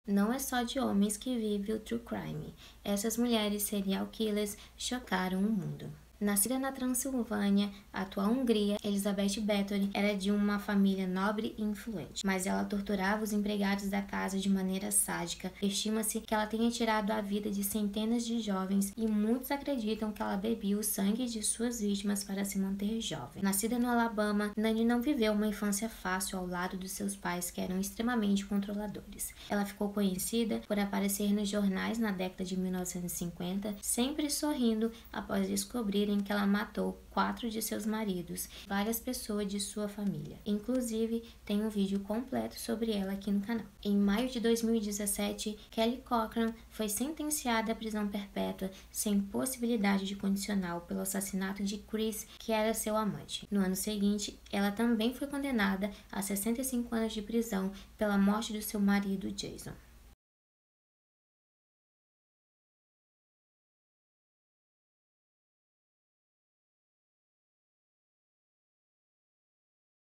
Mulheres que são serial killers
Nem só de homens vive o true crime. Conheça algunas mulheres serial killers.